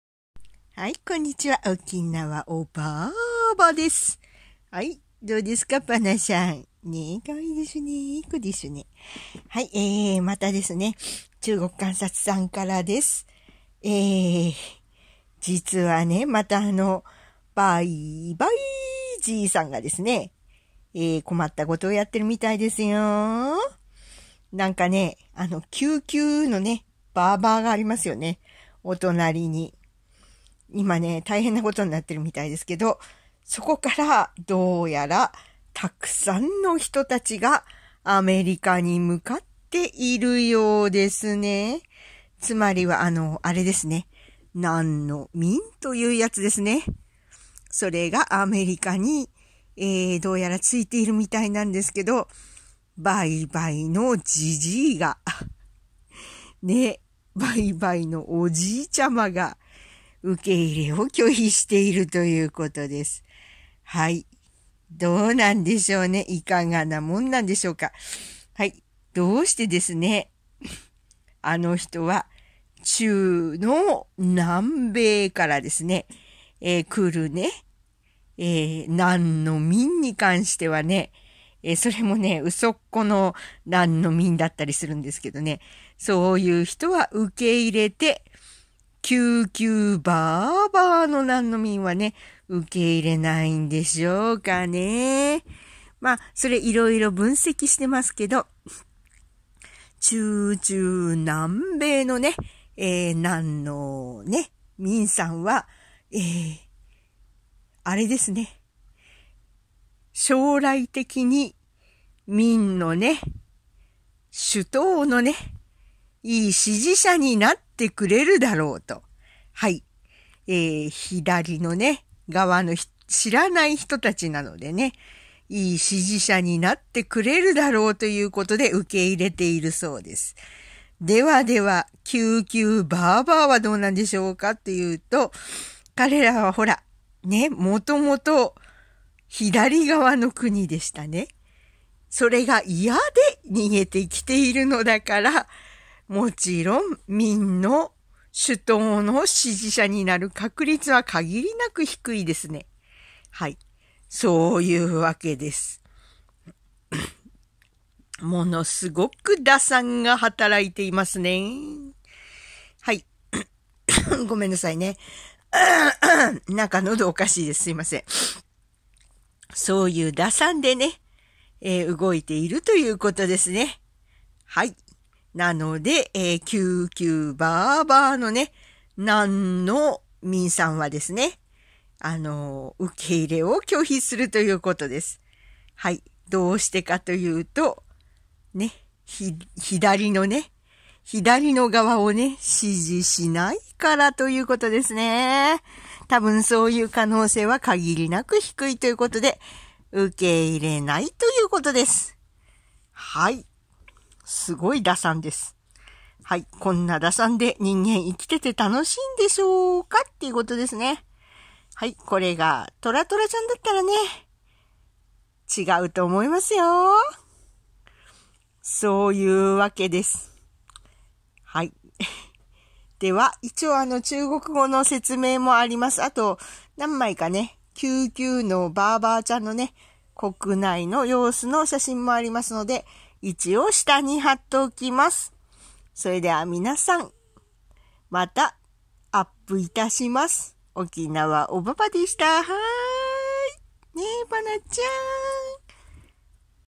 中国観察さんより・民の受け入れバイおじいちゃまの打算（概要欄から飛べます）
まあ、そういうことです。
きゅ～～ばーばーからの民は彼らにとってうまみが少ないんでしょうね。

しかし、そういう観点からしか物事を見れないとは心が貧しい人たちです。
まあ、心が貧しいから、とらさんをねたむんでしょうけどね。